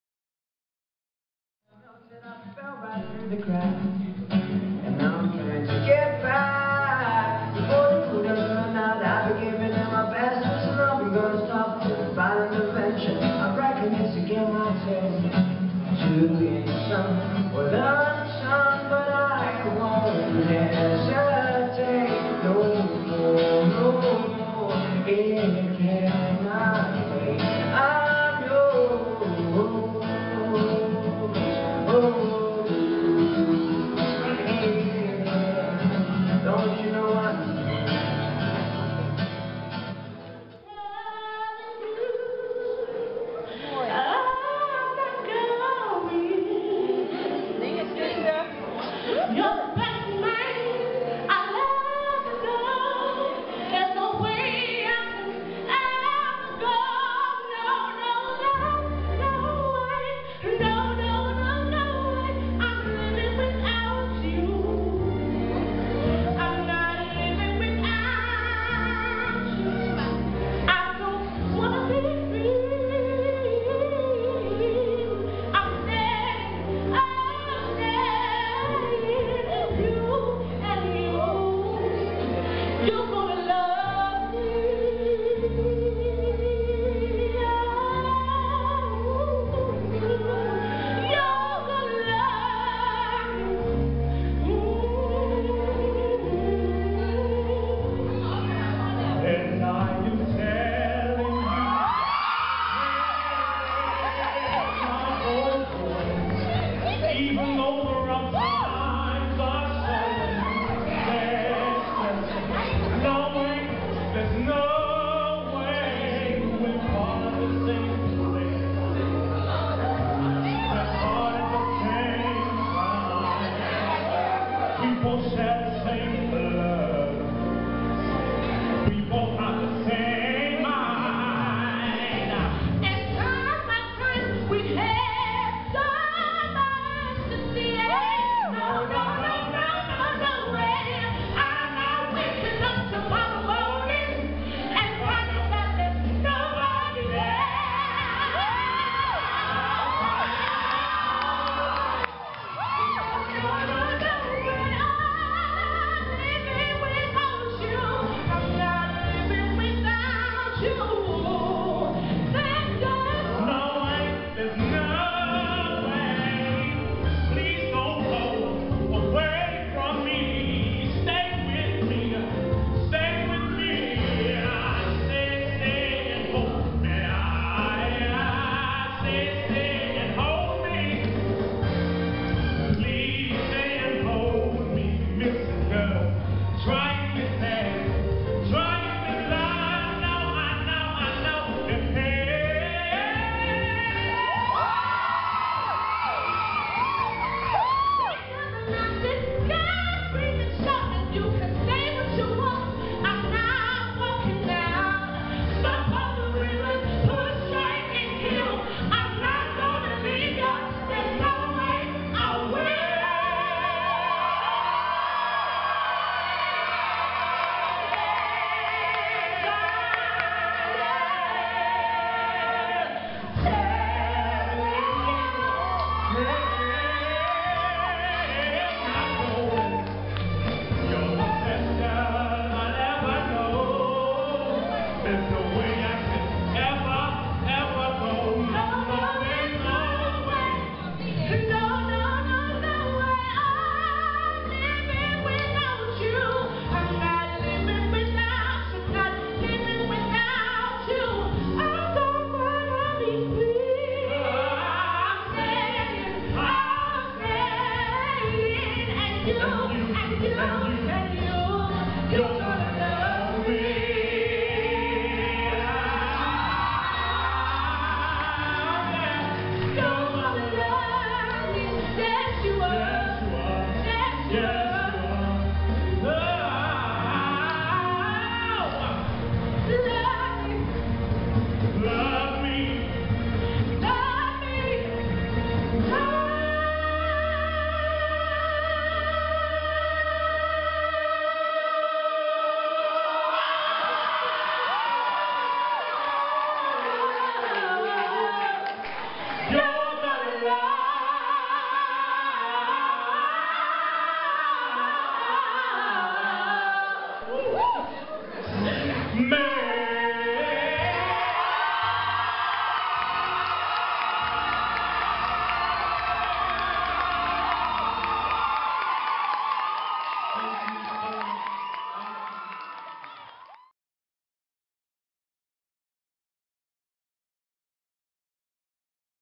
Music Performance at Sir George Monoux College
Music students performed a lunchtime concert in The Zone.
Come and see these students and more performing live at Monoux Live, Thursday 17th May 2012, 7pm at St. Mary's Church in Walthamstow Village. Tickets £2.